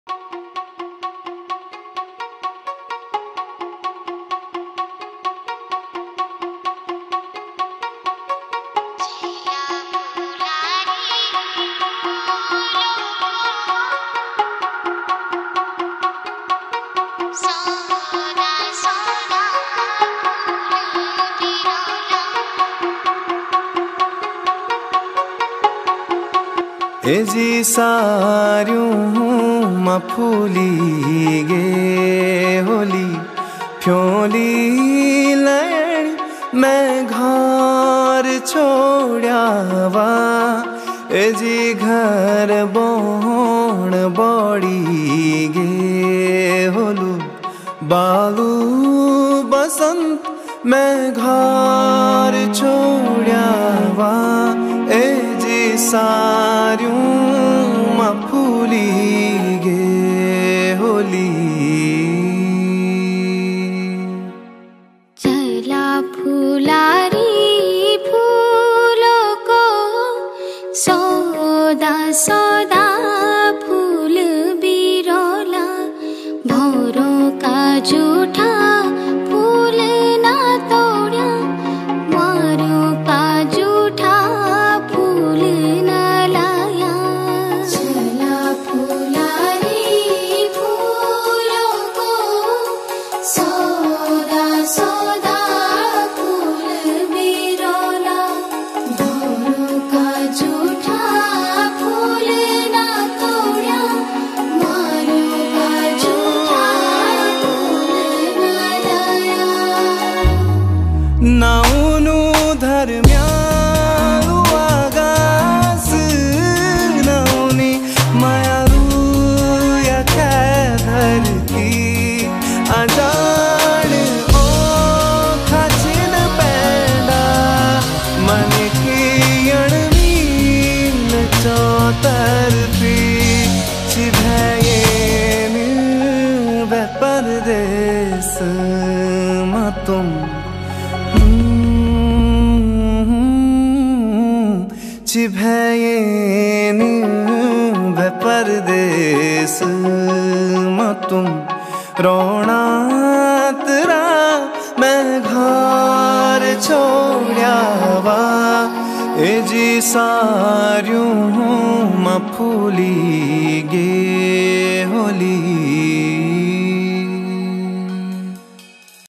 PHULARI THE FLOWER GIRL | PRAKRITI SINGH | BALA G LIVE TV | RISHIKESH
Plucking flowers and sharing the smell and blossoming buds along with smile to every doorstep every morning for an entire month of spring (chaitra- when flowers bloom) is a beauty of garhwal, introducing... as our own fulari (the flower girl) Prakriti Singh💐🍁🌺🌸🥀💐🌷
Address :
111/1 , Johar Complex, Tilak Road, Rishikesh (UTTRAKHAND)